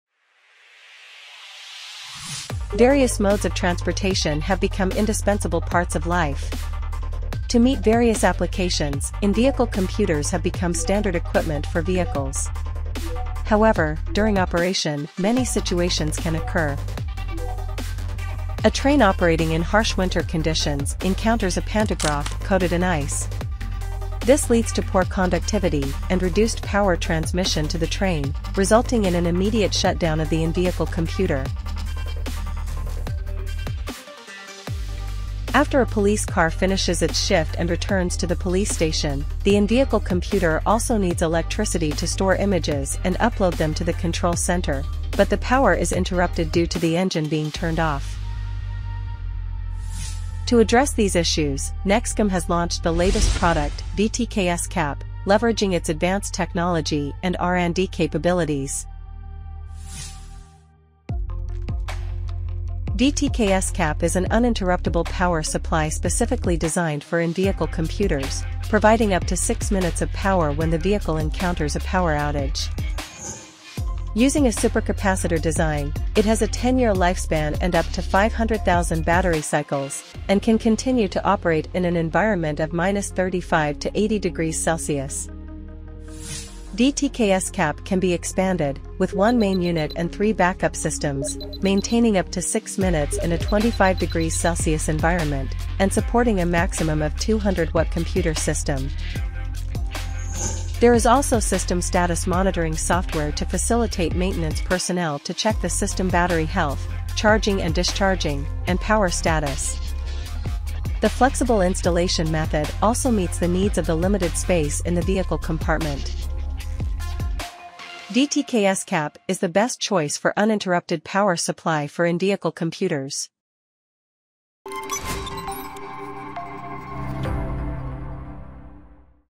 NEXCOM - VTK SCAP (English Version)
00:00 Opening
00:18 Application Scenarios
01:03 Product Features
01:58 Conclusion

NEXCOM's VTK-SCAP: A Reliable Power Solution for In-Vehicle Computers
VTK-SCAP is an uninterruptible power supply specifically designed for in-vehicle computers, providing up to 6 minutes of power when the vehicle encounters a power outage.

Key Features :
- Extended Battery Life: Utilizes supercapacitors for a 10-year lifespan and up to 500,000 battery
   cycles.
- Temperature Resilience: Operates in extreme temperatures from -35°C to 80°C.
- Scalability: Can be expanded with multiple units to support larger power requirements.
- System Monitoring: Comes with software for monitoring battery health, charging/discharging, and
     power status.